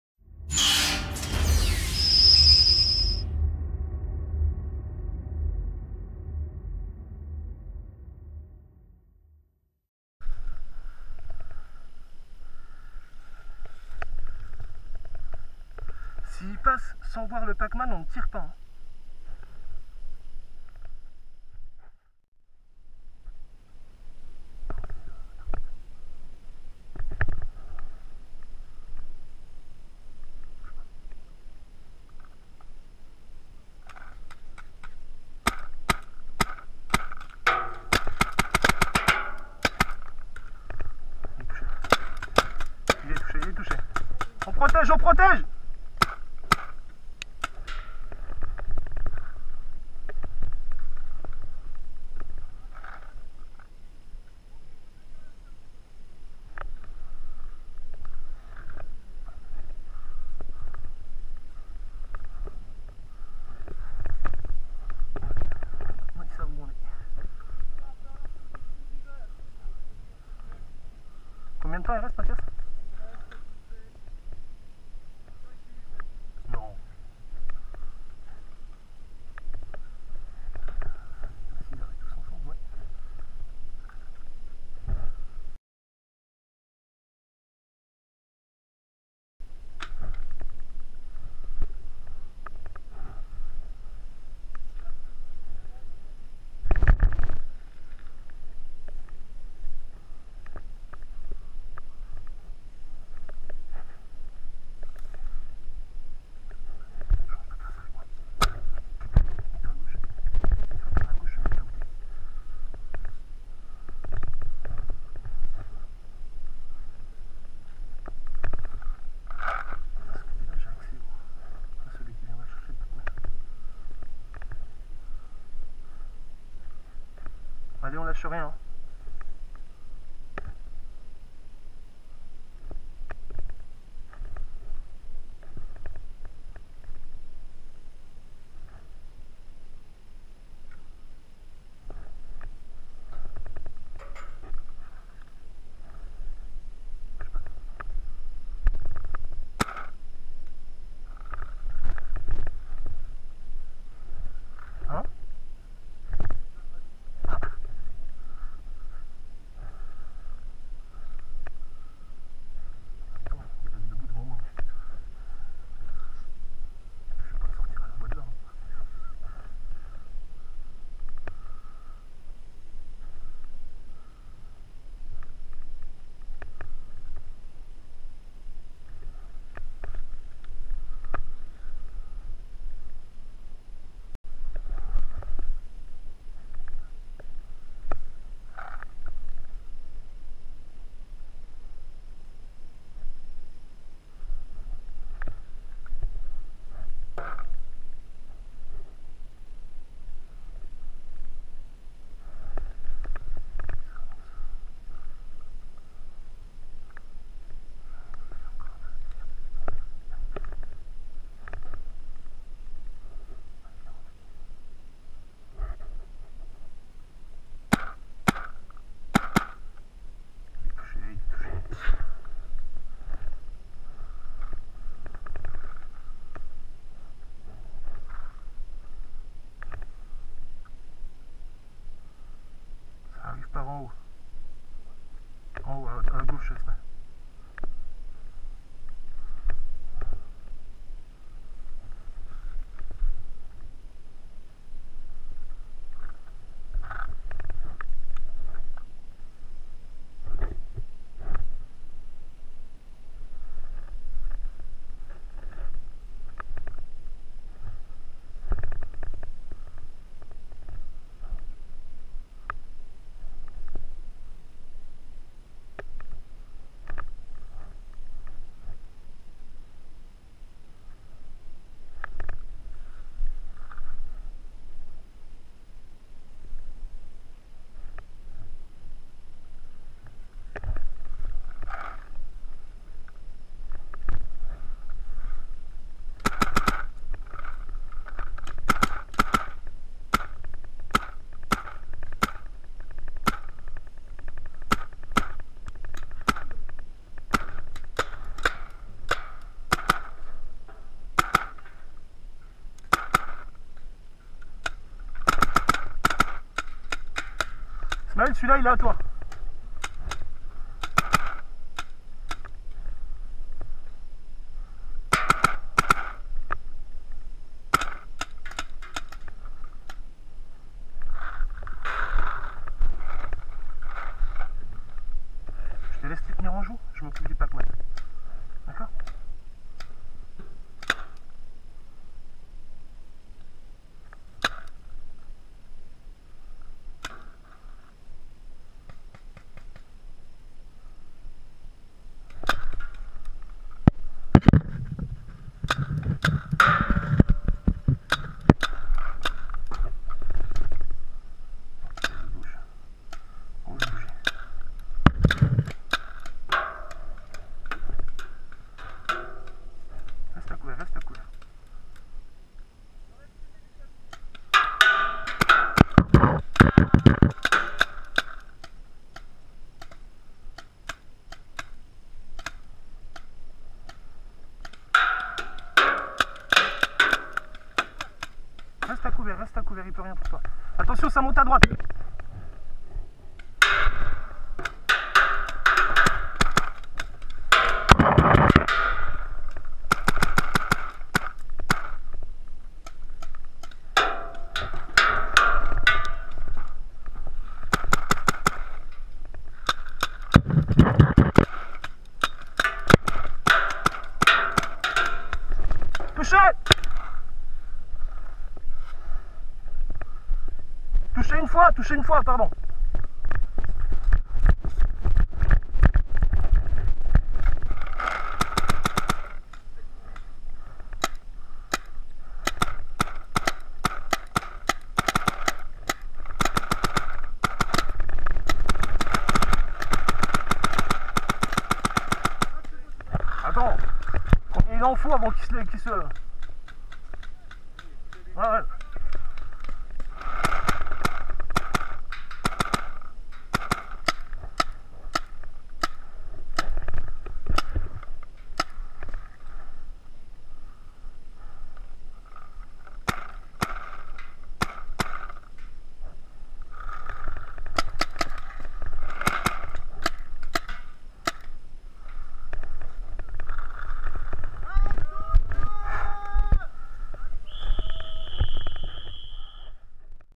Paintball du 070413- Village commando (54) - PacMan 2
Village commando à Bainville sur Madon (54)
Objectif : Défendre Pacwoman !
Config : Invert Mini + canon Freak + Loader Prophecy
Filmé avec une Gopro3 Black Edition en 1080p 30ips
Pour nous retrouver ou nous rejoindre :